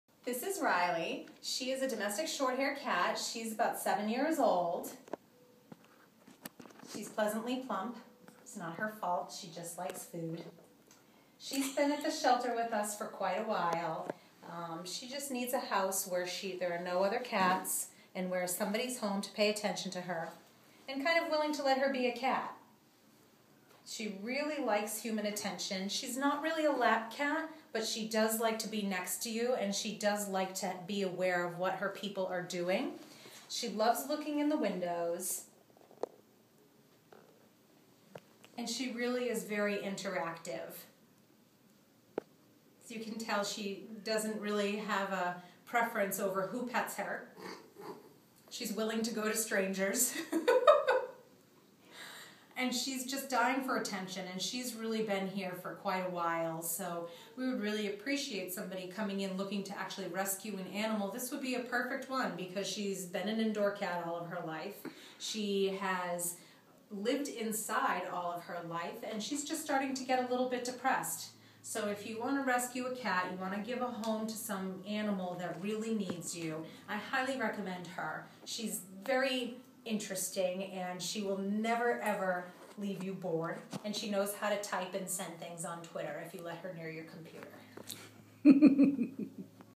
Meet Riley a Domestic Shorthair currently available for adoption at Petango.com! 9/14/2013 1:10:07 P
I am delighted to write this letter of reference regarding the possible adoption of Riley, also known affectionately by ARL staff as "Rye-Rye".  Riley has requested my assistance in helping her find a "just right" family for her.

I had the pleasure of having Riley as an office mate for several months.  When she first arrived at the shelter she had a lot of anxiety and we thought an office would feel more like a home to her.  And it apparently worked!  She did relax and was very affectionate and loyal.

She also tried to be helpful and even sent e-mails for me!  Though generally she did that before I had finished them...Again, she was just trying to help.

Though I recommend her without reservation, she does tend to like to be the center of your world.  If you aren't paying enough attention to her, she will let you know by giving you a little love nip!  I used a squirt bottle to get her to stop that so I would suggest using one if she gets a little demanding.  However, that being said, an experienced cat owner probably gets that but a first time cat lover could easily learn to do this.

Finally, if I could take Riley home, I would as she is adorable and smart.  As I have three cats and a big dog, Riley would be very uncomfortable and unhappy.  I really want her to be in a home where she isn't scared or worried even though I will miss her when she finds her "just right" family.

If you are considering Riley, I hope that this letter helps you in making a decision.

Sincerely,

Paula Mattis
President and CEO

PS—if Riley goes home with you, please ask to take her blue squirt bottle!